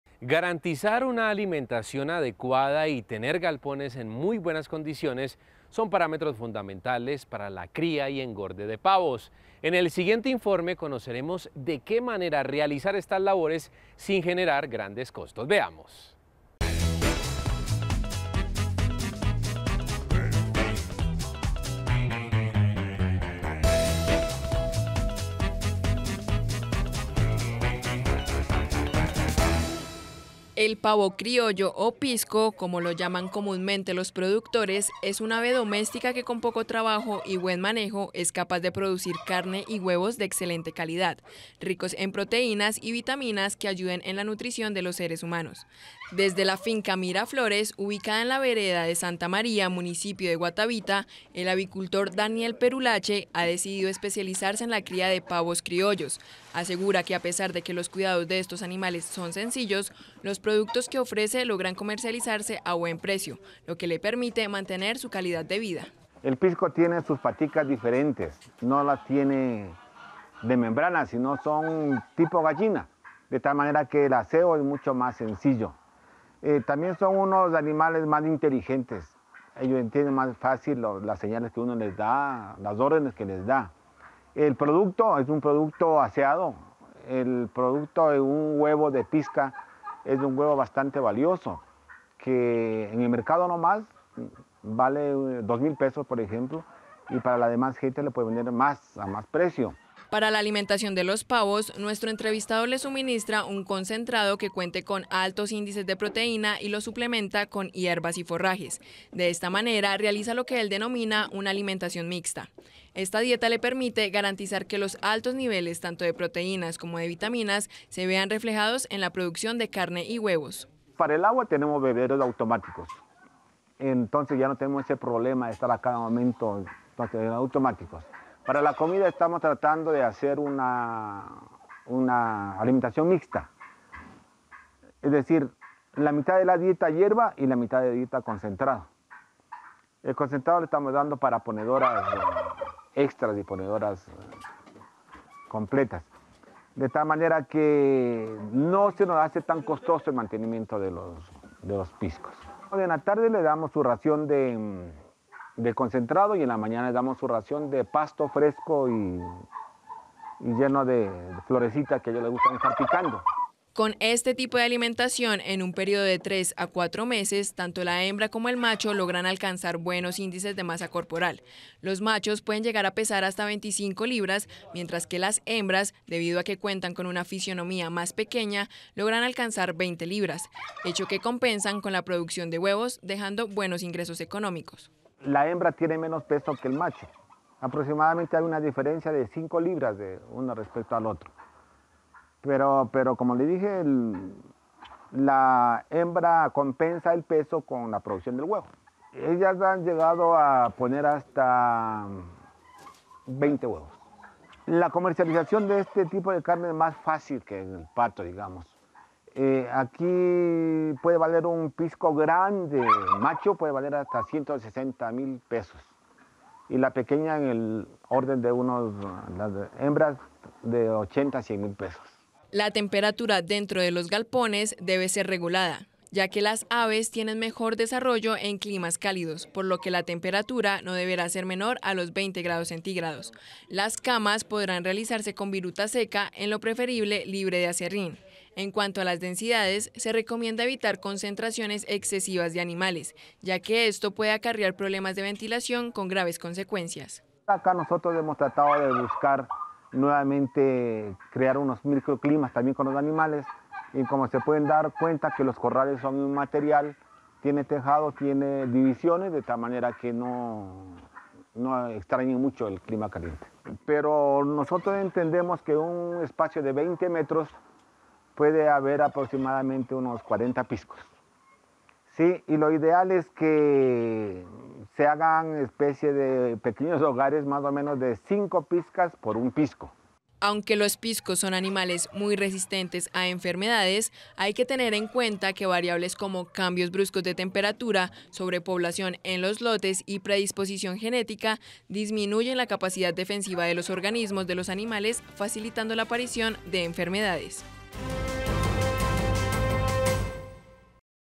Rendimientos y trabajo artesanal con pavos criollos
¿Cómo desarrollar la avicultura artesanal de los pavos criollos, mejor conocidos como piscos, para lograr buenos rendimientos? A continuación, se lo contamos.

Facebook: La Finca de Hoy

Bogotá – Colombia